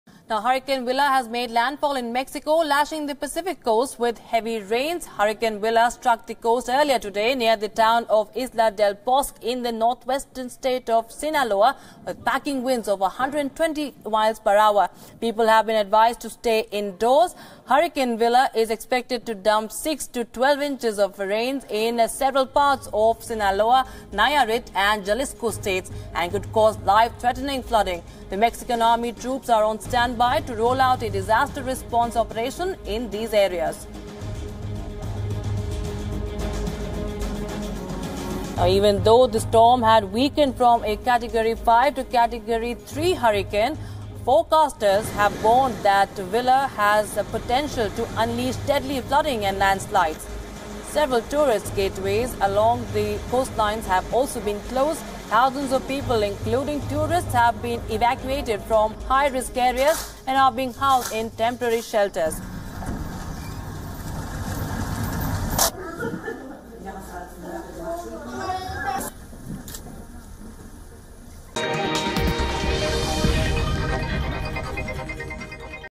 Hurricane Willa makes landfall in Mexico
Hurricane Willa has made landfall near Isla Del Bosque, Sinaloa, on the western coast of Mexico, approximately 50 miles south of Mazatlán.